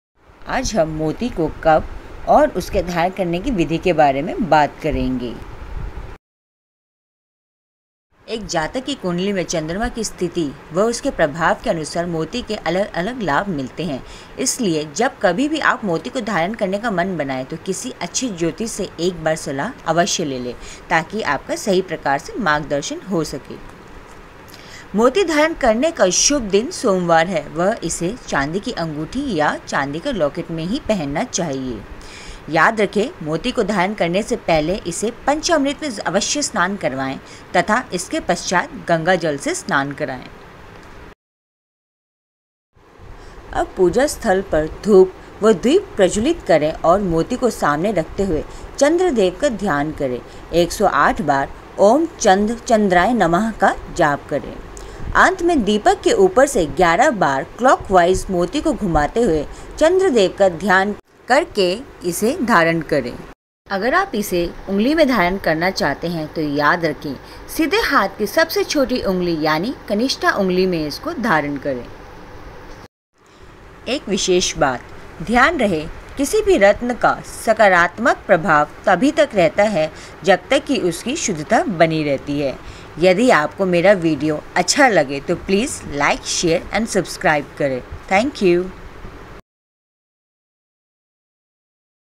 Moti dharan karne ki vidhi l Moti ko kaise dharan karein l Moti ko kis din dharan karna chahiye
मोती रत्न को शुक्ल पक्ष के किसी भी सोमवार के दिन चांदी की अंगूठी में बनाकर सीधे हाथ की सबसे छोटी ऊंगली में पहनना चाहिए।
इसे धारण करने से पूर्व दूध-दही-शहद-घी-तुलसी पत्ते आदि से पंचामृत स्नान कराने के बाद गंगाजल साफ कर दूप-दीप व कुमकुम से पूजन करके ॐ सों सोमाय नम: या ॐ चं चन्द्राय नमः मंत्र को 108 बार जपने के बाद ही धारण करना चाहिए।